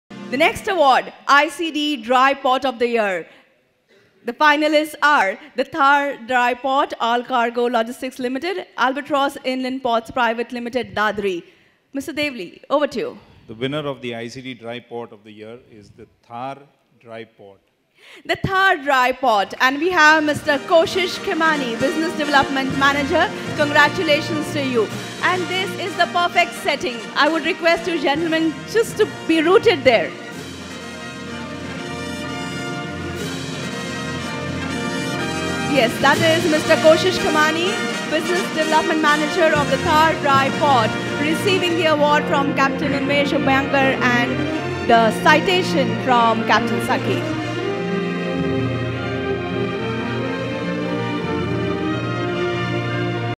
DRY Port of The Year -The Thar Dry Port ICD-DRY Port of The Year Winner,in SMA 2017
SamudraManthanAwards 2017 Ruby (Port) - The Thar Dry Port ICD-DRY Port of The Year Winner, Mr. Koshish Khimani, Business Development Manager, receiving the award for ICD-DRY Port of the year